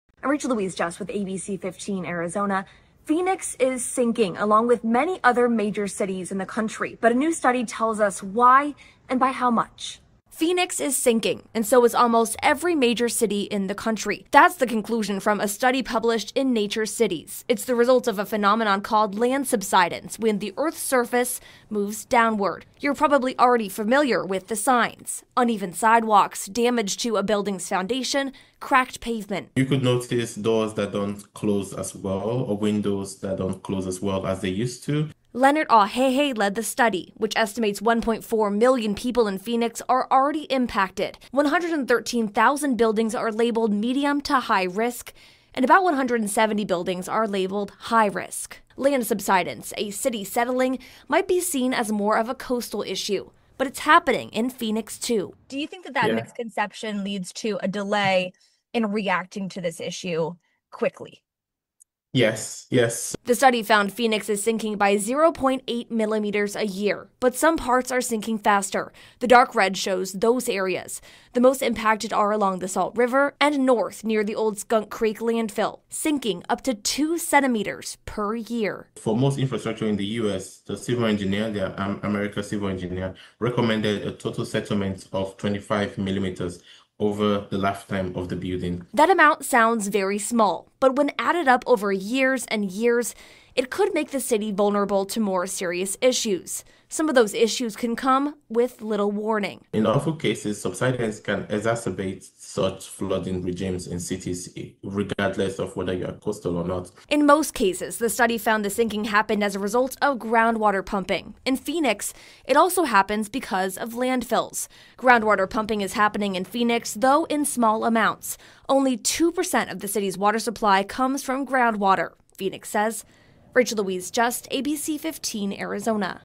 Phoenix is sinking, study finds over a million residents affected
Phoenix is sinking into the ground millimeter by millimeter, and researchers warn this gradual drop could lead to widespread infrastructure damage.  A new study published in Nature Cities reveals Phoenix is sinking by 0.8 millimeters per year, with some areas dropping even faster at up to 2 centimeters annually. The research indicates this isn't just a Phoenix problem - almost every major city in the country is experiencing similar land subsidence.